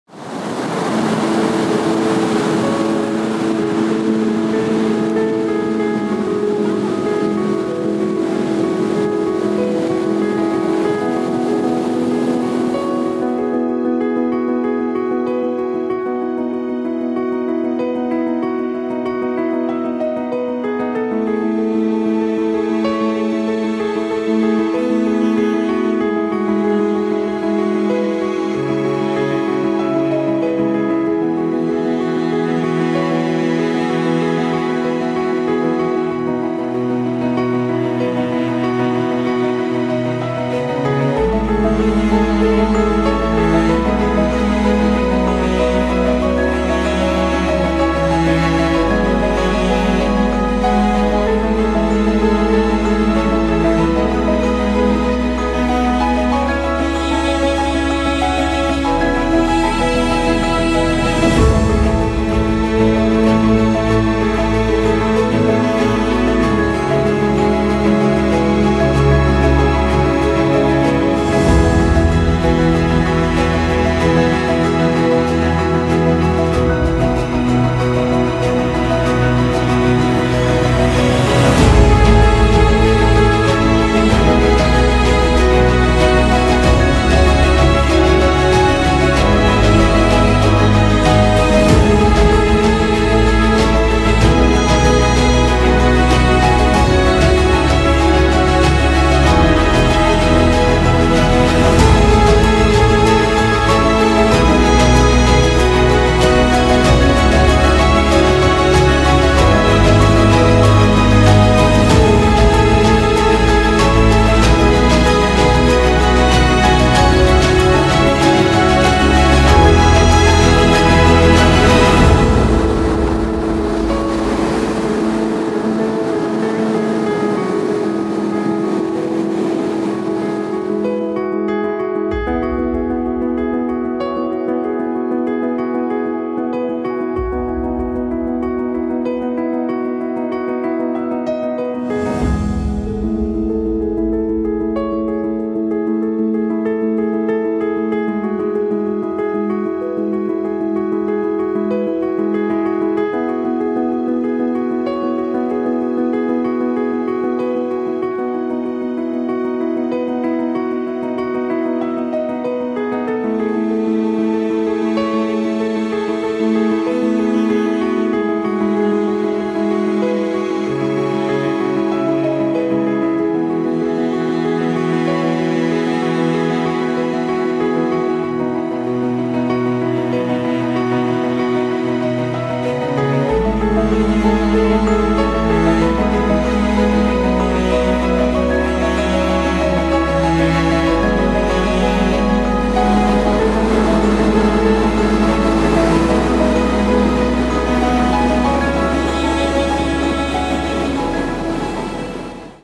Costa del Sol El Salvador
Video grabado en el 2018 en Playa Costa del Sol, hermosa playa ideal para disfrutar en familia.